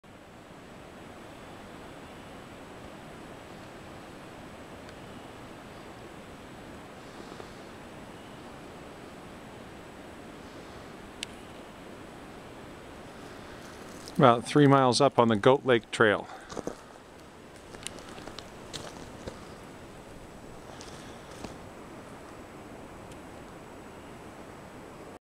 Sheep Mountain Range
About 3 miles up on the Upper Elliott Creek Trail (before the junction to Goat Lake).